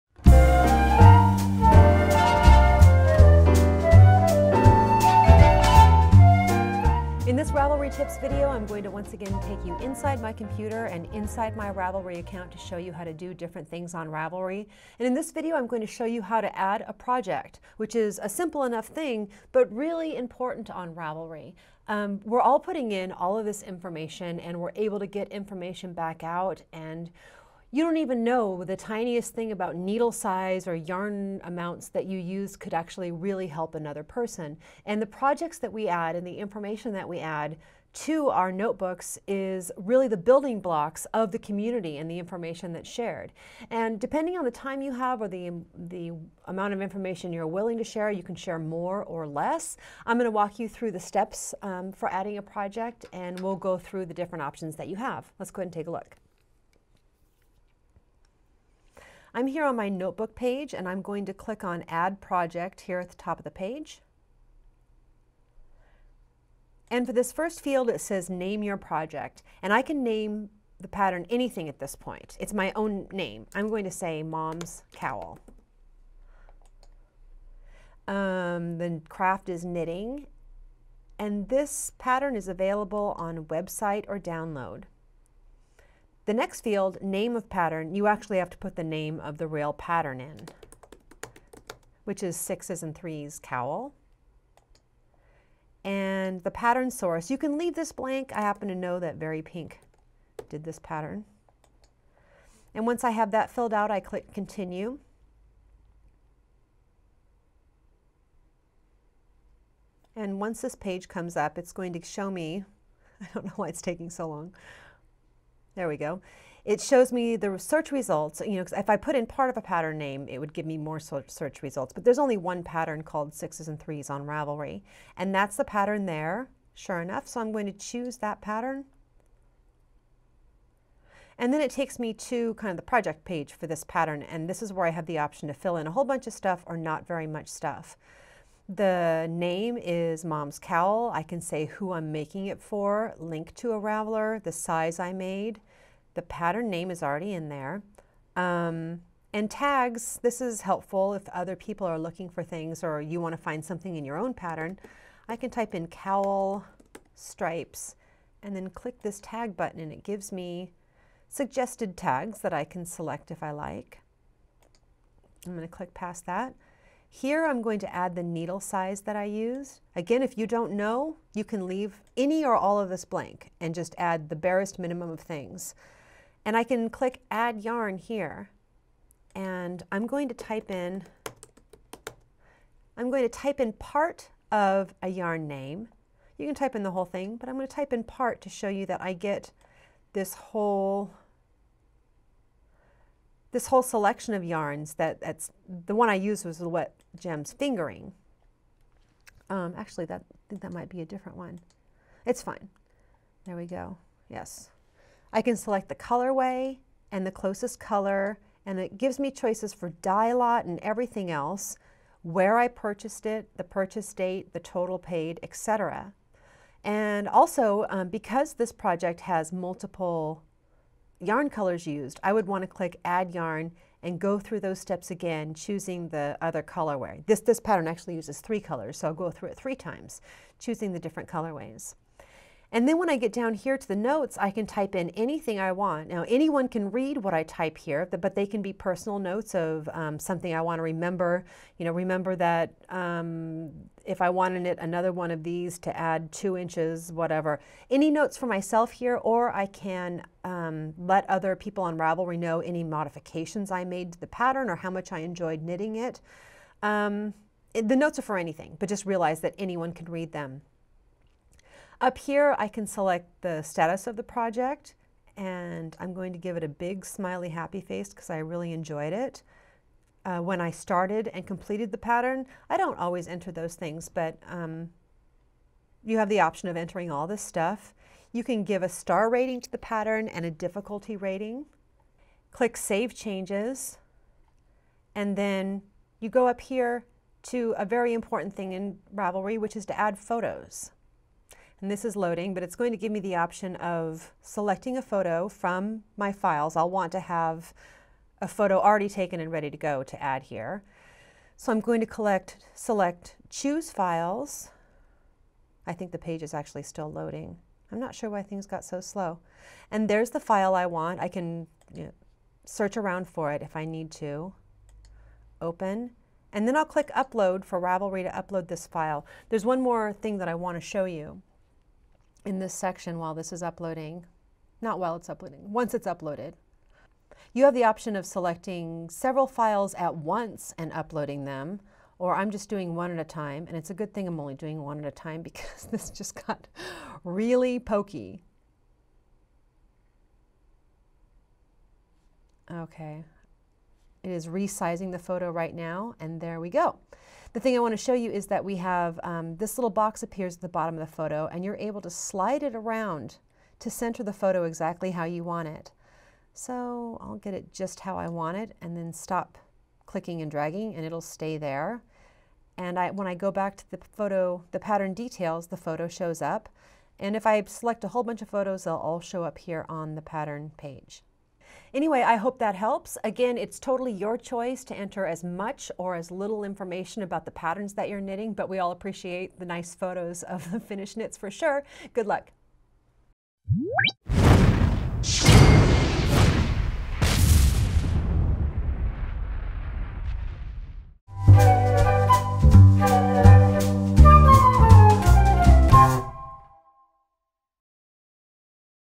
Ravelry Tips Adding a Project
Adding your knitting and crochet projects to Ravelry is the most basic and important way to contribute to the Ravelry community.  You can add as little or much information as you like, from notes to yourself to photos for the world to see.  In this video, I walk you through the basics of adding a project to your Ravelry notebook.

Information on things you'll see in this video:

The pattern I used as an example of a Ravelry project is my Sixes and Threes Cowl, pattern + video tutorial:

If you would like to friend me on Ravelry, I'll friend you back!  You can find my profile here:

We have a VeryPink Knits group on Ravelry, feel free to join the discussion: